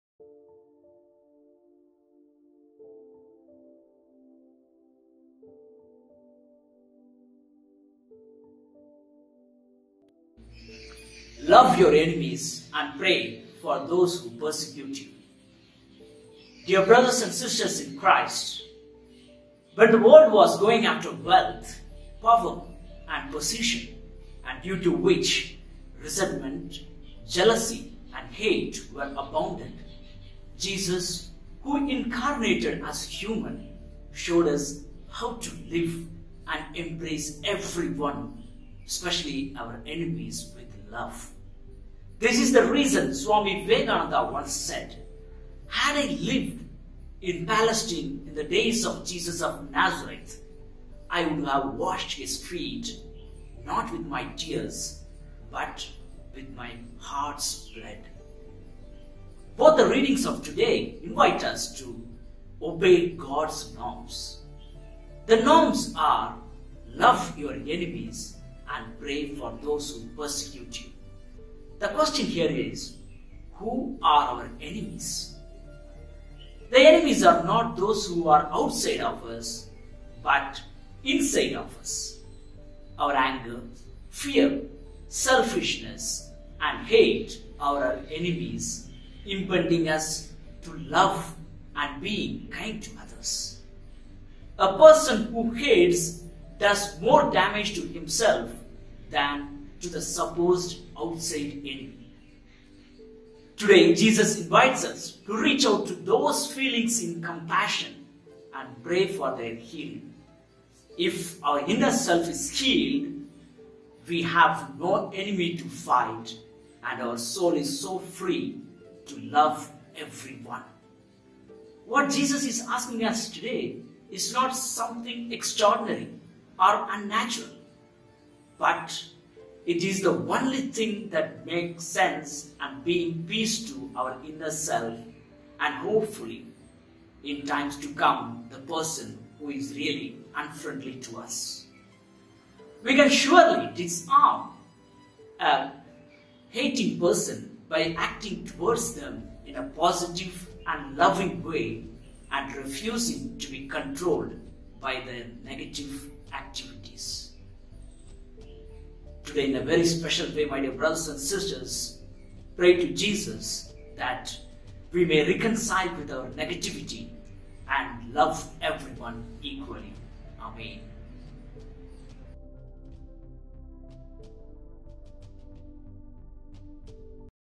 LOVE YOUR ENEMIES by Pravin Jose, S.J.
Reflection on the Gospel passage (Matthew 5: 43-48)